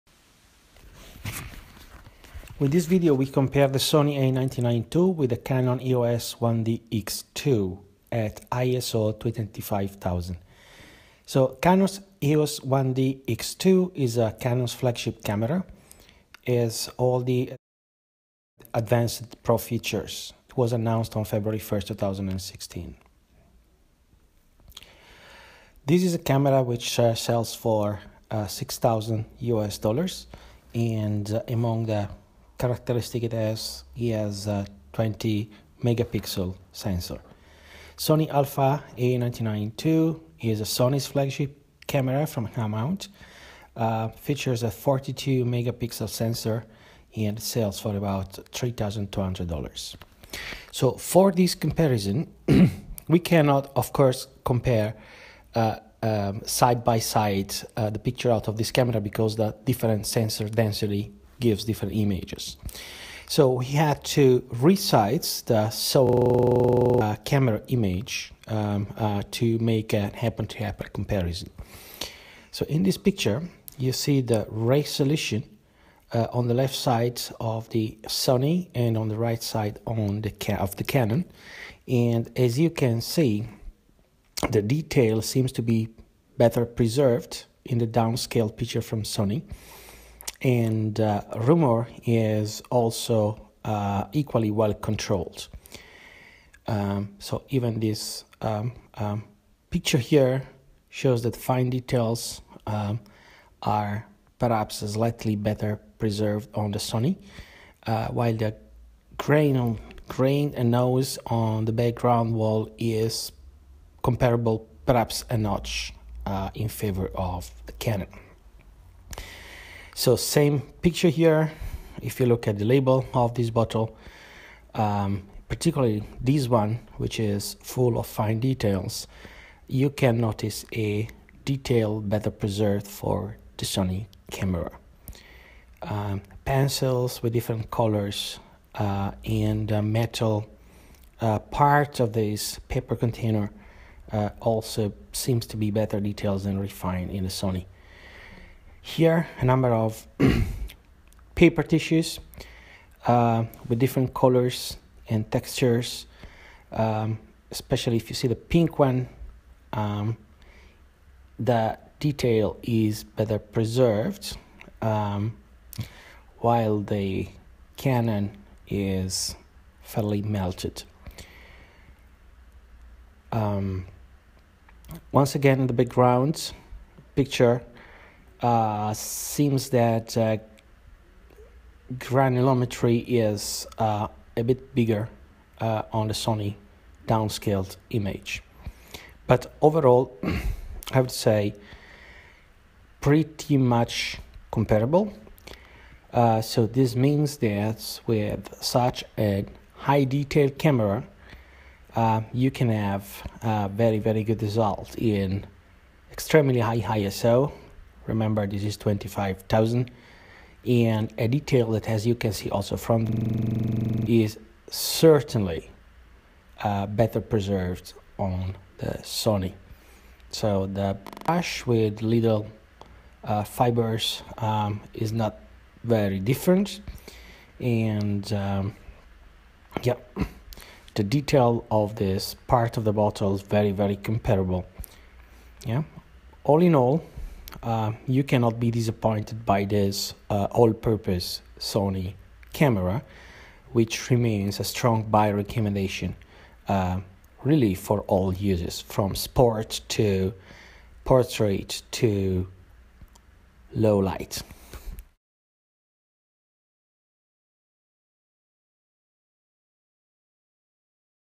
Sony A99ii vs. Canon EOS 1 DX Mark ii at ISO 25600
Comparison of High ISO performance (25600 ISO) of Sony A99ii versus Canon EOS 1 DX Mark ii flagship camera.
There is a price difference here of $ 3200 (Sony) vs $6000 (Canon), and people refer to the Canon as the absolute leader in low light, among professional FF DSLRs.

When pictures from Sony A99ii are downscaled to allow for a fair comparison with the EOS1DX Mark ii, noise is fairly comparable but Sony is better in preserving the detail.

In summary,  if an image from a high megapixel sensor is scaled down to the same resolution as a lower megapixel sensor of the same physical size, the result is that noise is averaged out of the image from the scaling better than the improved signal strength of the larger pixels reduces it.

(Pictures used were taken from Imaging Resource, , they make a meritory service to the photographers' community and I want to thank them & invite you to visit their website)